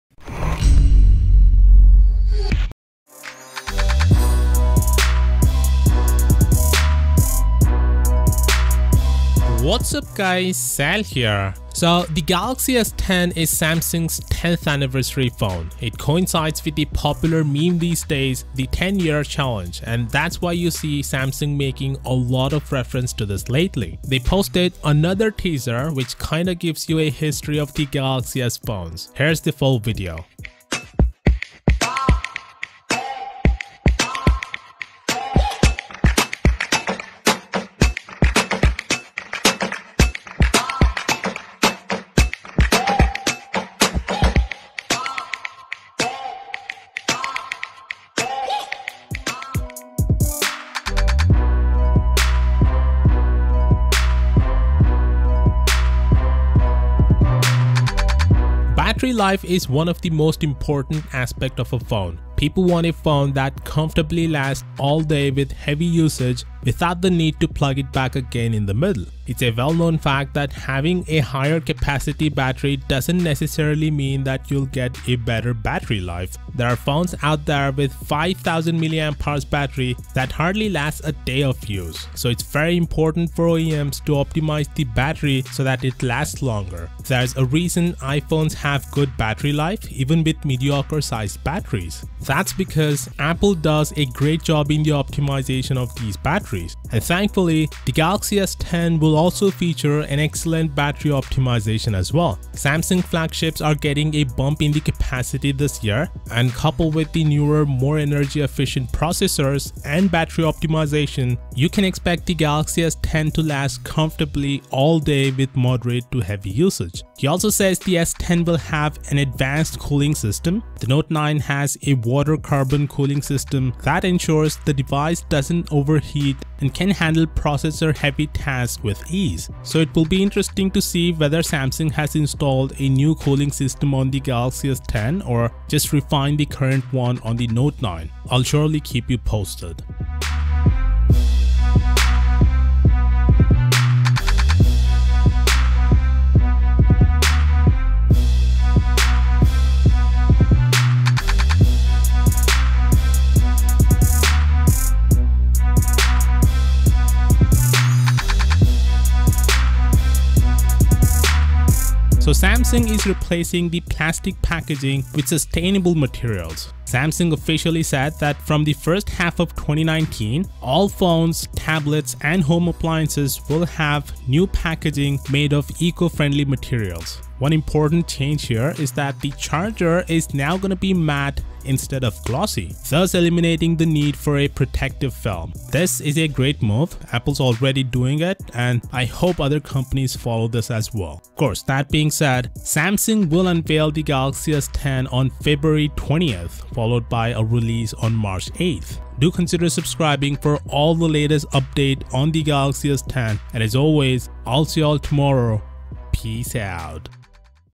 Samsung Galaxy S10 - OFFICIAL TEASER 2
Samsung Galaxy S10 will launch in under a month and Samsung has released another official teaser for the device. Samsung Galaxy S10 leaks suggest the device will have an infinity o display. Samsung Galaxy S10 rumors also say the device will be released on March 8th.